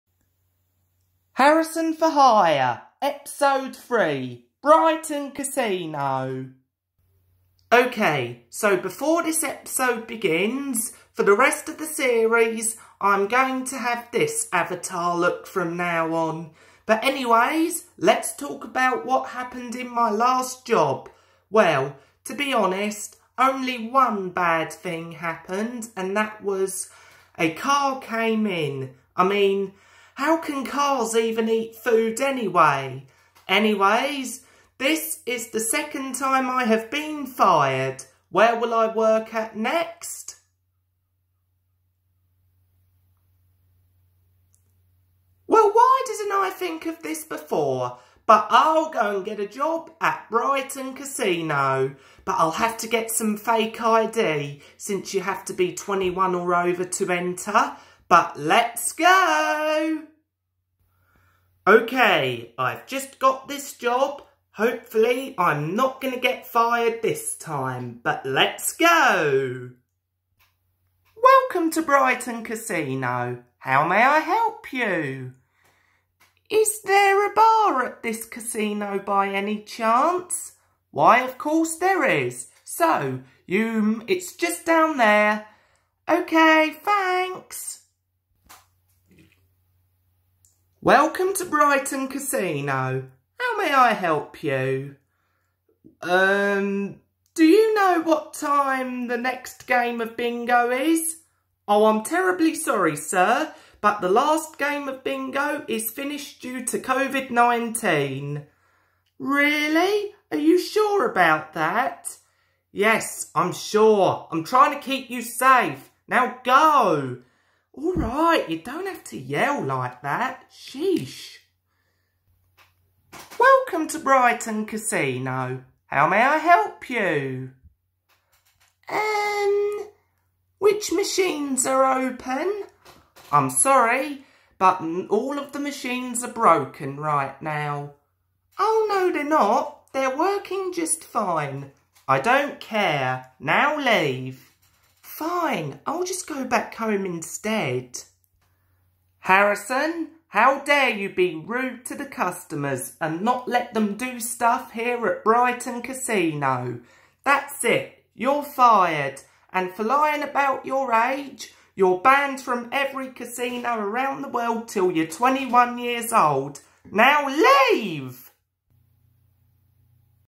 Harrison For Hire #3: Brighton Casino
Welcome to the 3rd episode of Harrison For Hire! After getting fired at Burger King, I decide to go and get a job at Brighton Casino!